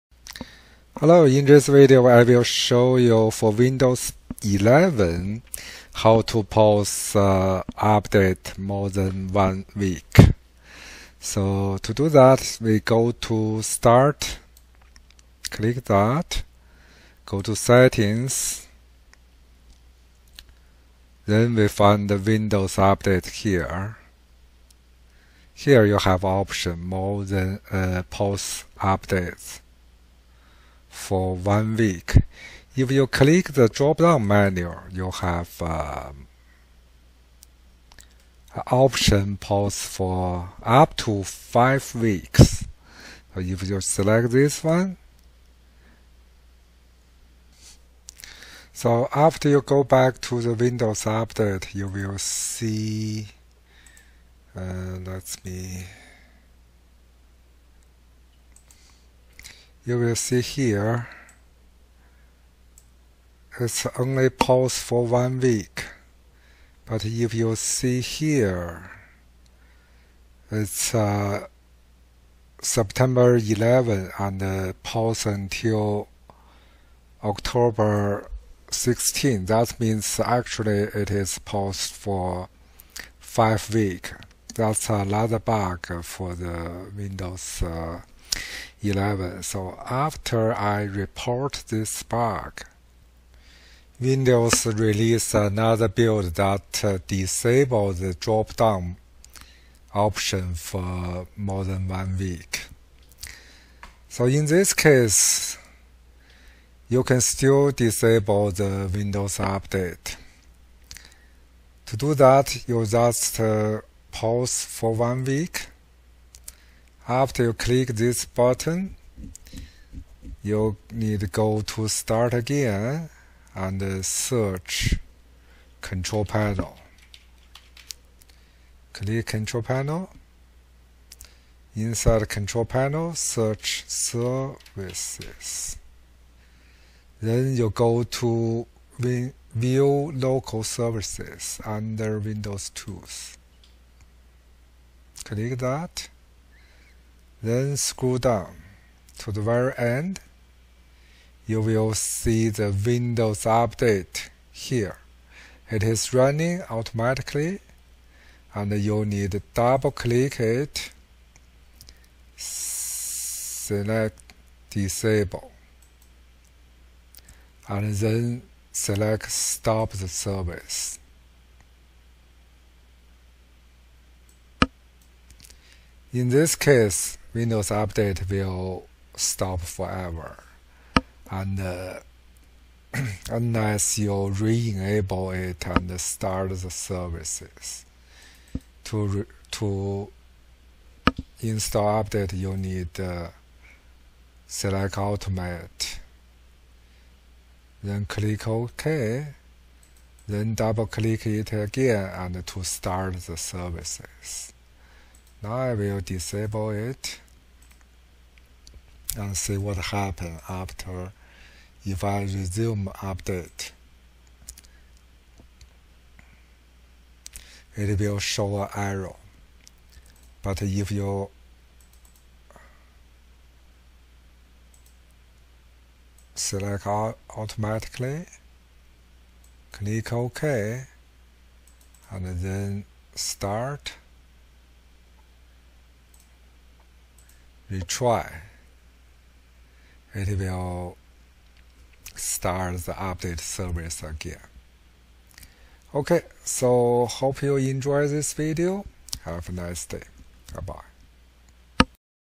windows 11 pause update more than 7 days
Windows 11 basics: How to pause and disable updates
1. Open Settings (WIn+I).
2. Click on Windows Update on the left side.
3. Select Pause for 5 weeks

Or to Disable Windows 11 update:
After fellow the steps that pause windows 11 update.
Open control panel, search "services"
Double click windows update services.
change the startup type to: disabled
Stop the Windows 11 update services.